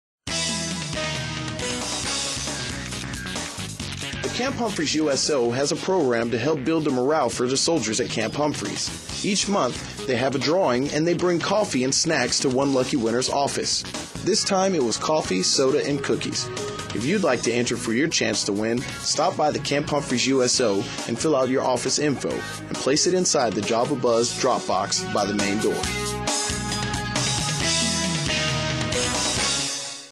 USO Java Buzz - Camp Humphreys, South Korea - 120907
The Camp Humphreys USO has a program to help build morale for Soldiers at Camp Humphreys. Each month they have a drawing and they bring coffee and snacks to one lucky winners office. This time it was coffee, soda and cookies. If you'd like to enter for your chance to win drop by the Camp Humphreys USO and fill out your office info and place it inside the java buzz drop box by the main door.